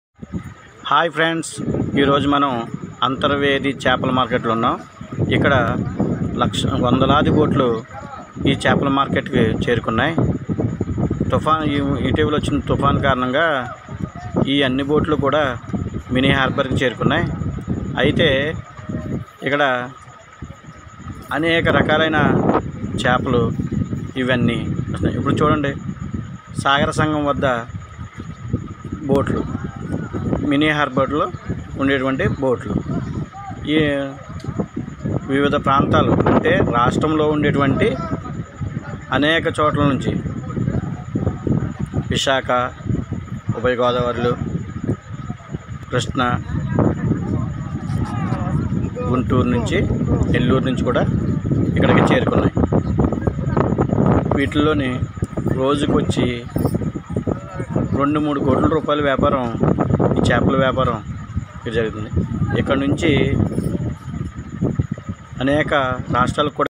అంతర్వేది చేపల రేవులో బోట్లు
అంతర్వేది చేపల రేవుకి అధిక సంఖ్యలో చేరుకున్న బోట్లు.